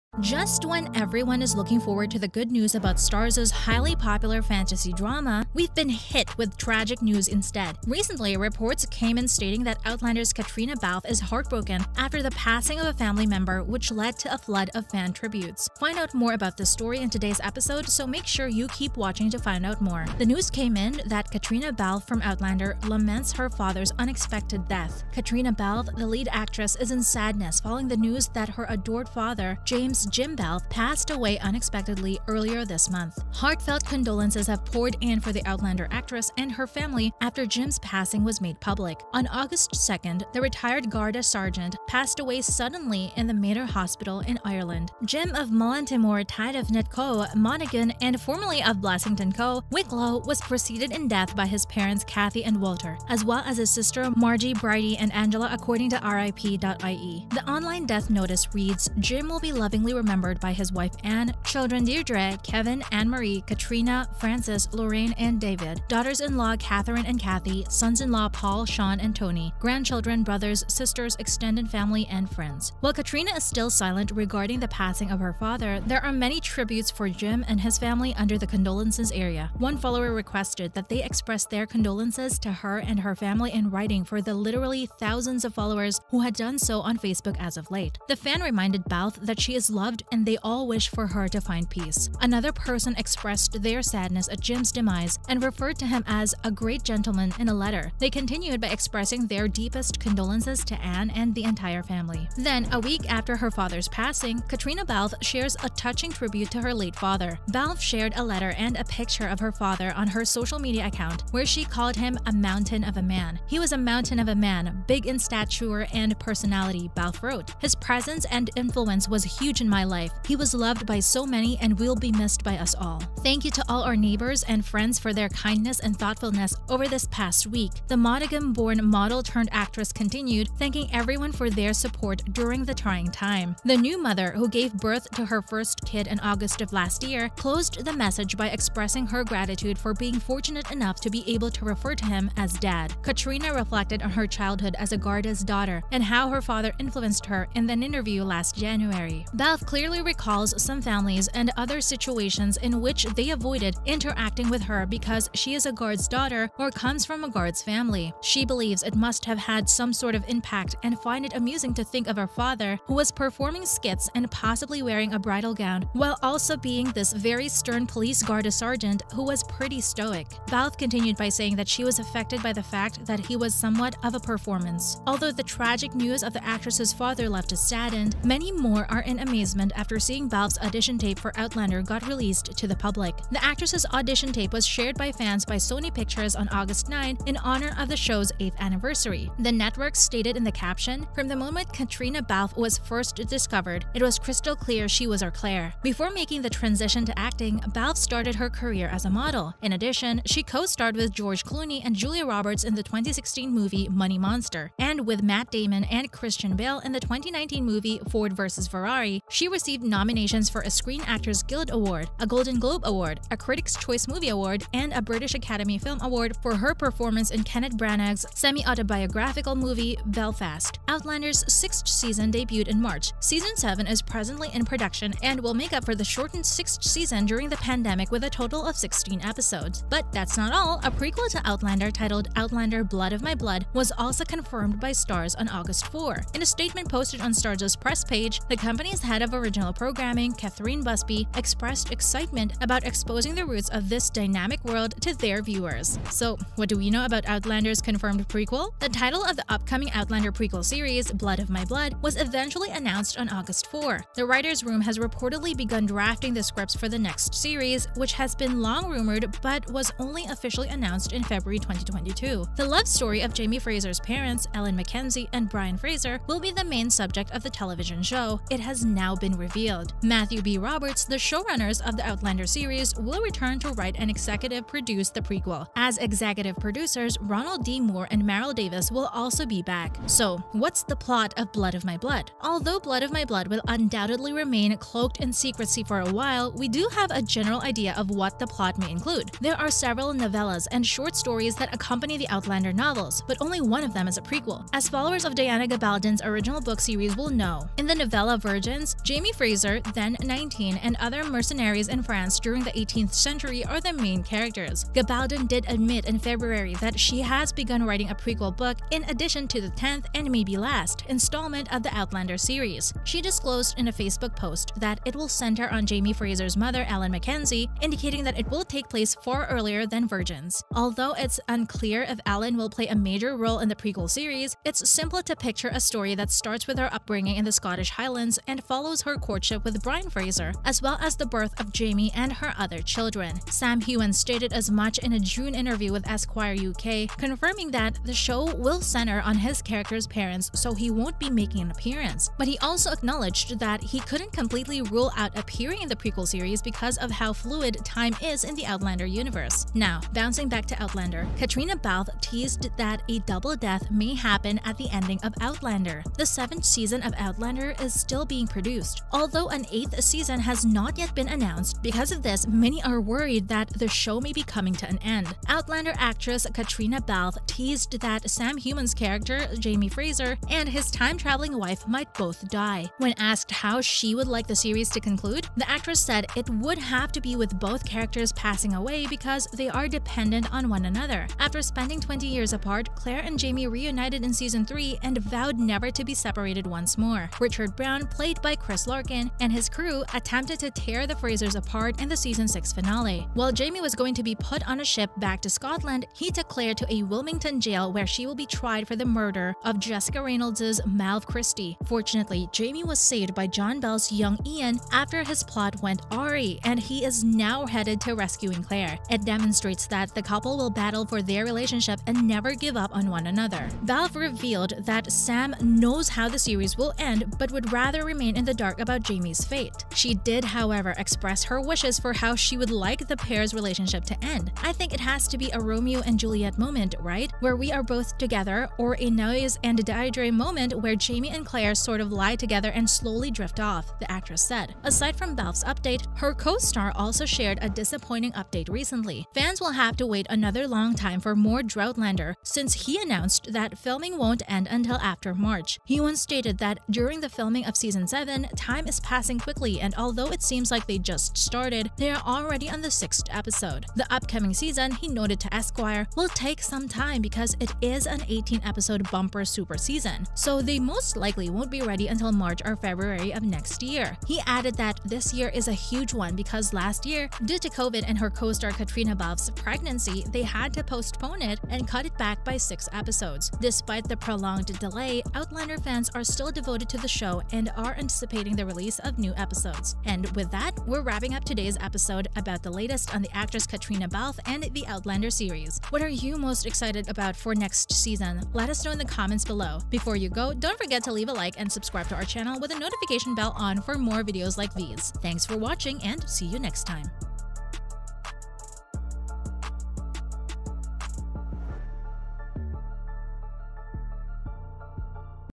Outlander Star HEARTBROKEN After HORRIFIC Passing Of Family Member..
Welcome back to Film Savvy. Caitriona Balfe is in mourning after it was reported that her beloved father, James 'Jim' Balfe, suddenly passed away this week. The Outlander star and her family have been inundated with heartfelt tributes after the news of Jim's death was revealed. The retired garda sergeant died "suddenly" in the Mater Hospital in Ireland on Tuesday. In January, Catriona recalled her upbringing as a Garda's daughter and how her father inspired her.

While Caitriona has yet to comment on her father's death, plenty of tributes flooded in for Jim and his family in the condolences section. One read: "Caitriona, and family, I write for literally the thousands of fans who have posted condolences to you and your family on Facebook at this time. You are loved, and we all wish for you to find peace. Meanwhile, speaking on the latest episode of Variety's Awards Circuit Podcast, the 42-year-old actress revealed that she fell pregnant "not longer after" the seven-week shoot for her upcoming film Belfast wrapped in November of last year. Caitríona and her music producer husband, Tony Gill, announced via social media on 18 August that they had welcomed their first child together, a son. They have chosen not to reveal his name.